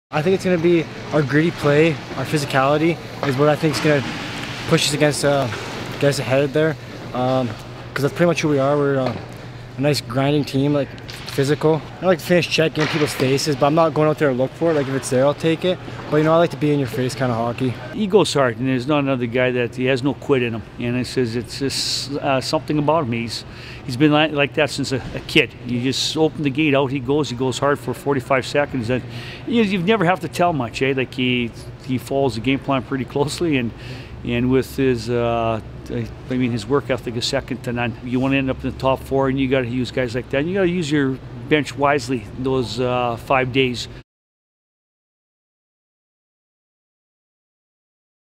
Contacts’ Telus Cup drive motors behind Dino Antoniadiss
Antoniadias, a hard-working winger on the West Region champion Contacts, has a motor that never stops. He is expected to be one of the inspirational leaders for the Contacts this week in New Brunswick at the 2016 Telus Cup national midget hockey championship.